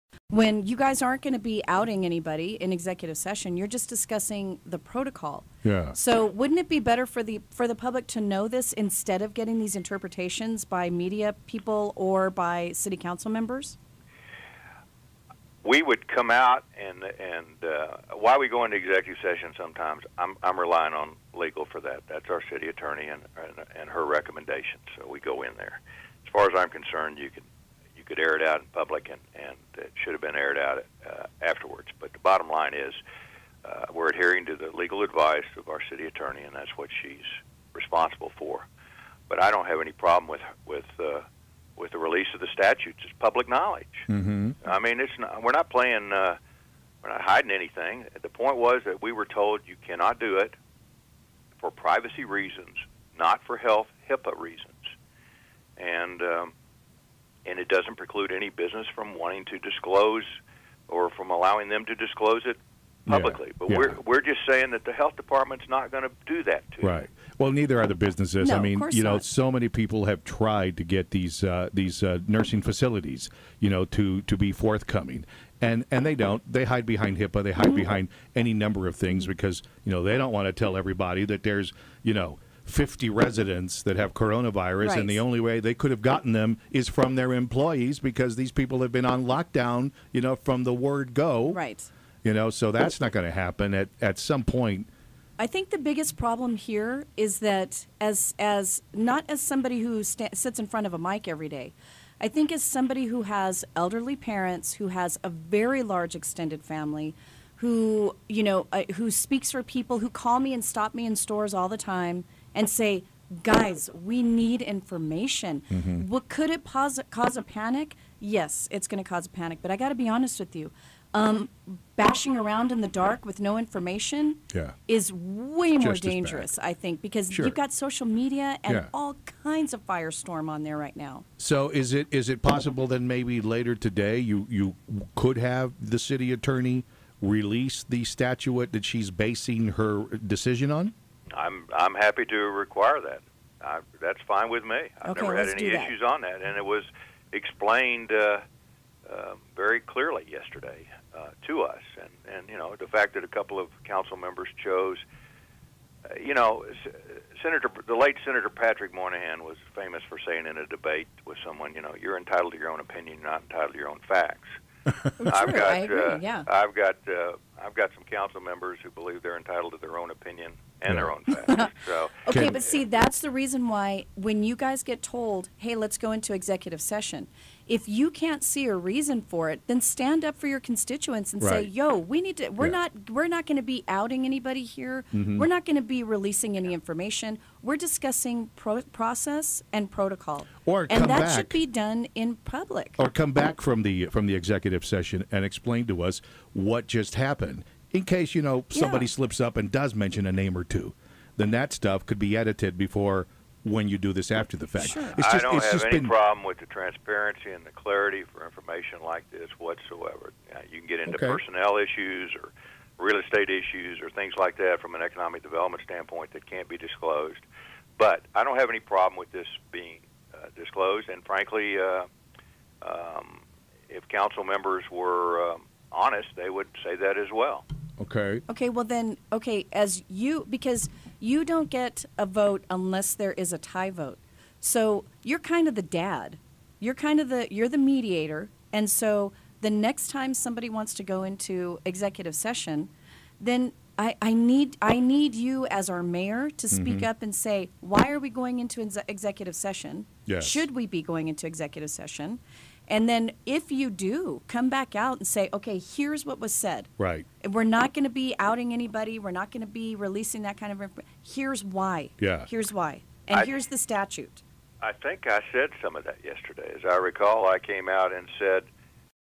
Mayor Margo Interview: Why the Need for Executive Session?
After a controversial vote, El Paso Mayor Margo speaks with Mike & Tricia about why they needed to go into executive session to discuss a vote on sharing COVID-19 clusters in El Paso.